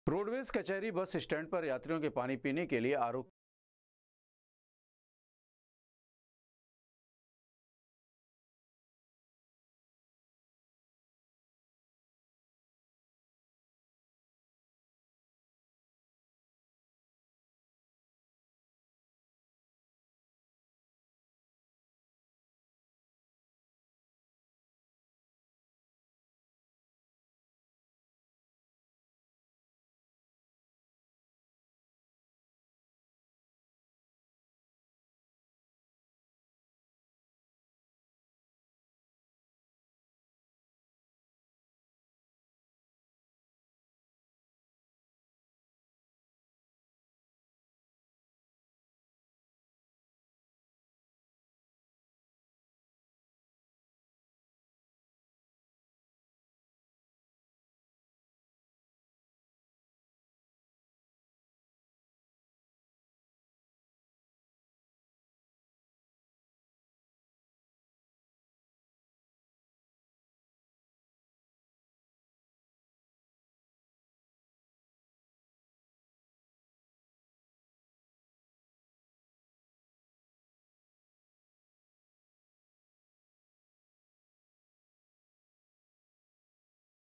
कचहरी बस स्टैंड पर यात्रियों के पानी पीने की आरओ की,की गयीं है ब्यवस्था,सहायक क्षेत्रीय प्रबंधक अशोक
कचहरी बस स्टैंड पर यात्रियों के पानी पीने की आरओ की,की गयीं है ब्यवस्था,सहायक क्षेत्रीय प्रबंधक अशोक कुमार सिंह ने दी जानकारी